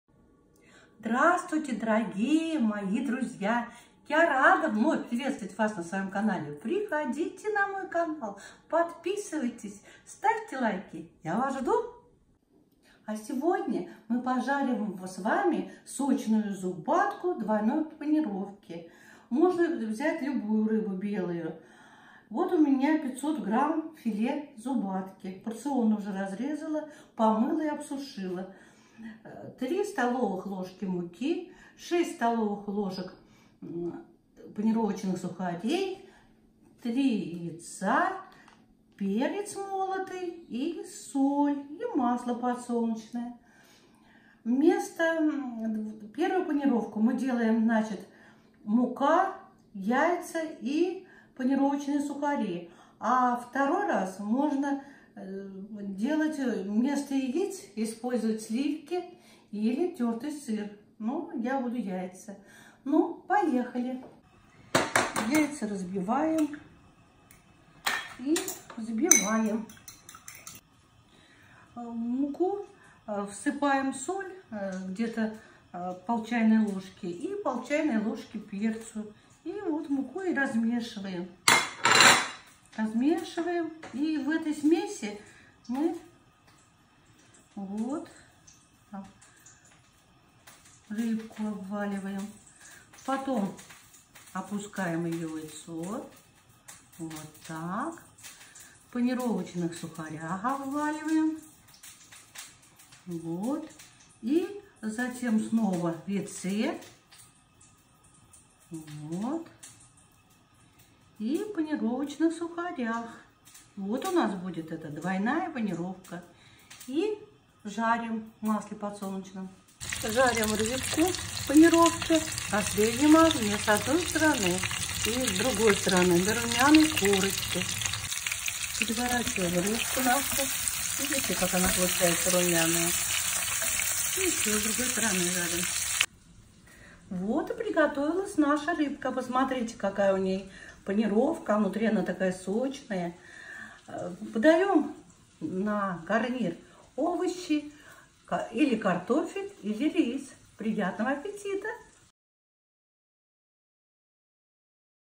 Белая рыбка в двойной панировке
Приготовление рецепта несложно. Вкусный простой рецепт приготовления белой рыбки. Двойная панировка делает рыбу сочной и нежной, так как она запечатывает рыбу.
Очень хороша с картофельным пюре.
Состав продуктов
1. Рыба (филе зубатки, можно использовать филе любой белой рыбы)
2. Яйца 3 шт
3. Панировочные сухари 6 ст. л
4. Мука 3 ст. л
5. Соль, перец молотый
6. Масло подсолнечное
Этапы приготовления
1. Филе помыть, разрезать порционно, обсушить.
2. В муку всыпать  по 1/2 ч. л соли и перца, размешать.
3. Обвалять сначала  в муке затем во взбитых яйцах и панировочных сухарях.
Затем опустить в взбитые яйца и обвалять в панировочных сухарях.
4. Жарить на раскалённой сковороде с добавление масла на среднем огне с двух сторон до румяной корочки.
Подаём с овощами, рисом и картофельным пюре.
Приятного аппетита!

Ссылки на мои видео:
•салат Цезарь
https:/wFvkHV_3fJs/youtu.be/